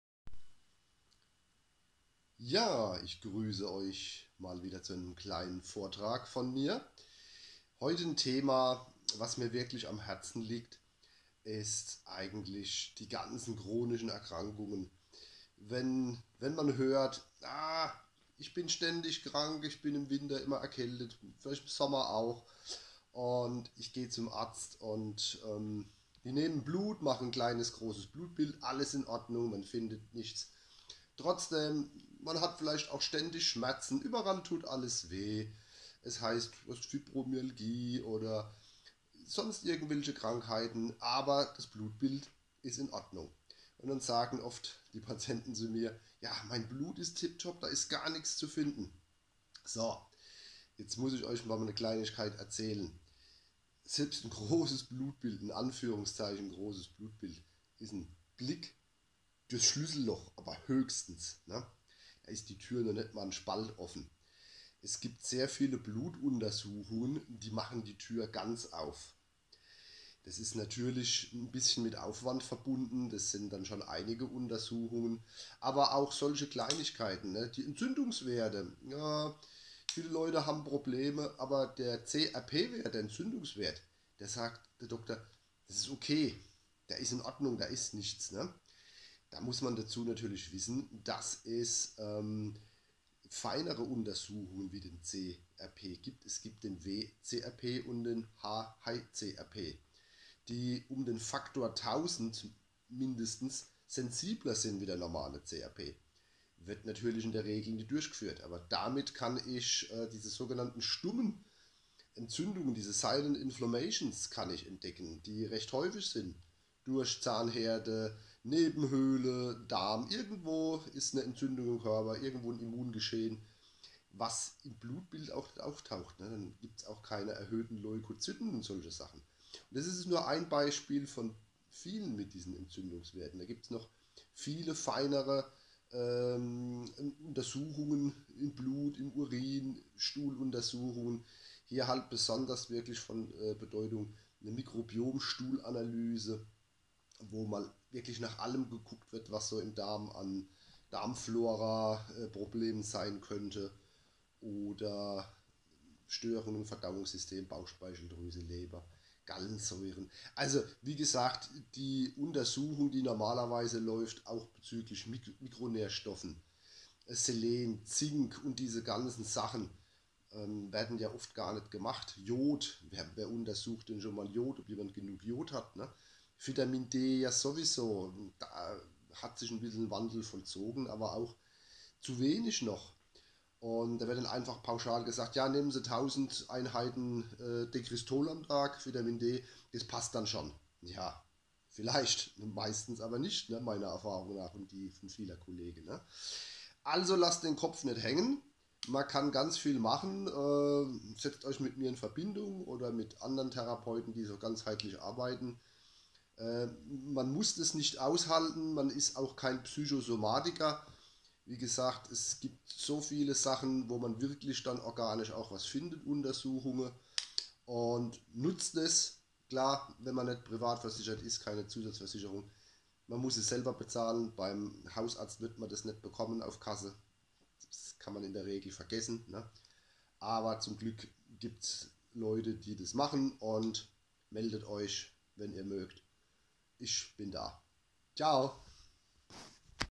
Grosses Blutbild ist normal, aber Du fühlst dich trotzdem krank?
Blutbild beim Arzt ist normal, er sagt, Dir fehlt nichts aber du hast Beschwerden. Was kannst Du tun?